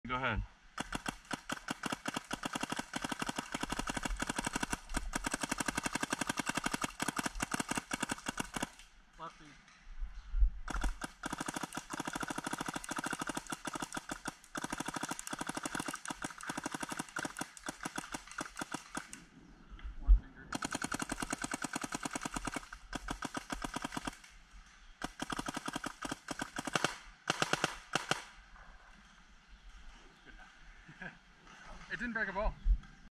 MG100 Dye Box Rotor Shooting
Raw Shooting Video
Gun:         Planet Eclipse MG100
Loader :   DYE Box Rotor
Barrel:      Inception Designs XL insert Barrel
Tank:        Immortalite 77/45
Camera:   Hero7
Misc:        Deadly Wind Hair45 Valve, Inception Designs Fang Trigger.